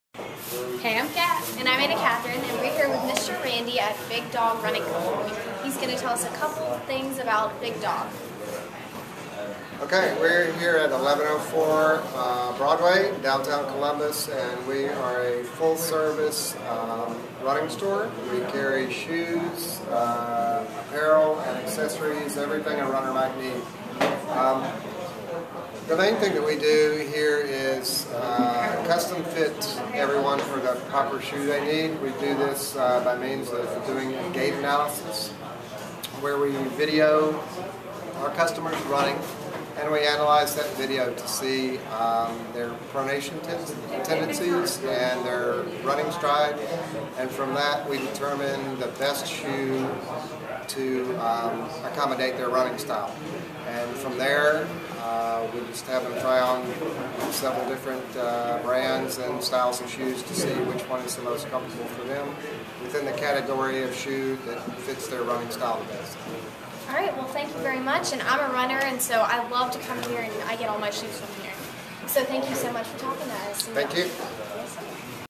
Big Dog Running Company (Uptown Columbus)
Like to run? This is THE place to get shoes perfect for your feet - an excellent place for the entire family!